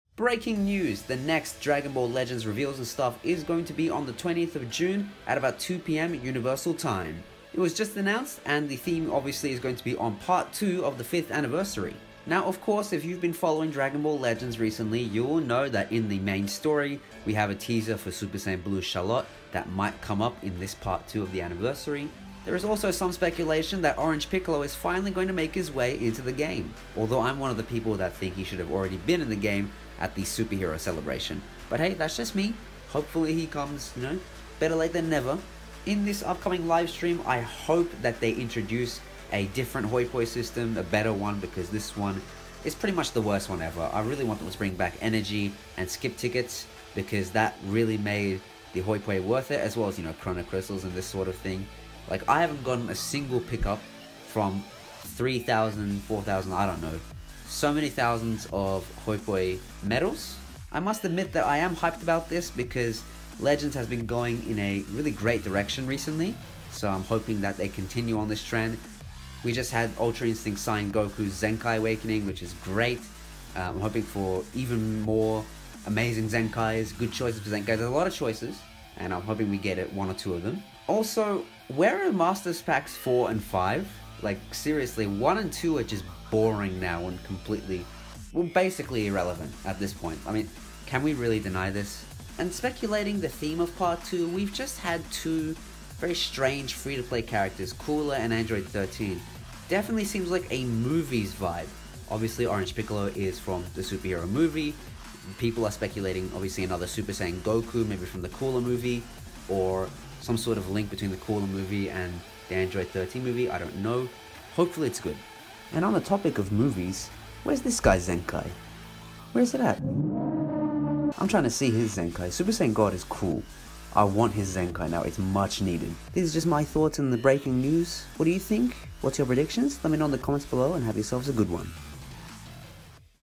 BREAKING! REVEALS AND STUFF PART 2 ANNOUNCED! Dragon Ball Legends 5th Anniversary
Reveals and Stuff is on the 20th of June, at 2 pm UTC! WHO WILL BE THE FEATURED CHARACTER? ORANGE PICCOLO? SSJ GOKU?